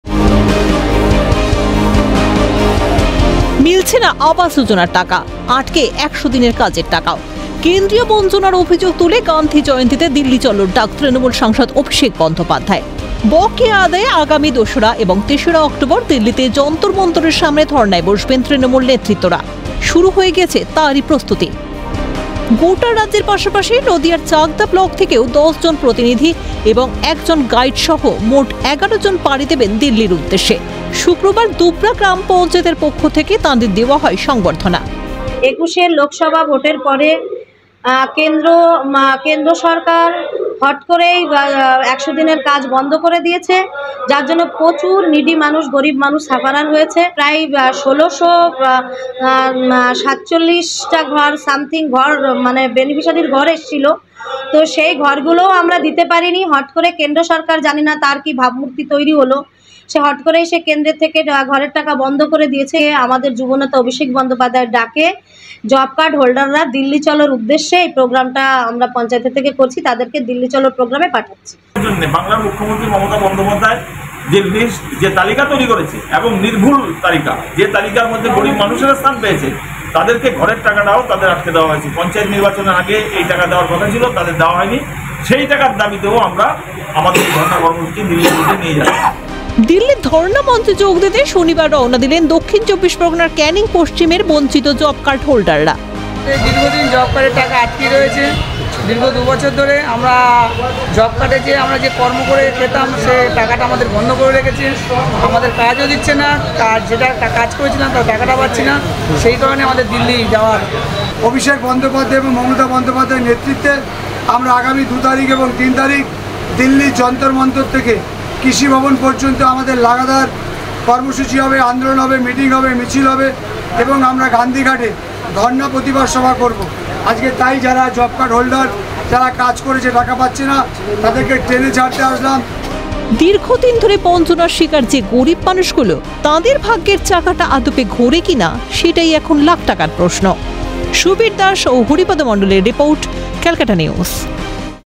অভিষেকের ডাকে দিল্লির পথে বঞ্চিত জব কার্ড হোল্ডাররা । ক্যালকাটা নিউজ
অভিষেকের ডাকে দিল্লির পথে বঞ্চিত জব কার্ড হোল্ডাররা

গান্ধী জয়ন্তীতে দিল্লি চলোর ডাক তৃণমূল সাংসদ অভিষেক বন্দ্যোপাধ্যায়ের। গোটা রাজ্য জুড়ে চলছে তারই প্রস্তুতি। নদিয়ার চাকদা ব্লক থেকেও ১০জন প্রতিনিধি এবং ১জন গাইড সহ মোট ১১ জন পাড়ি দেবেন দিল্লির উদ্দেশ্যে। পাশাপাশি রওনা দিলেন ক্যানিং পশ্চিমের বঞ্চিত জব কার্ড হোল্ডাররা।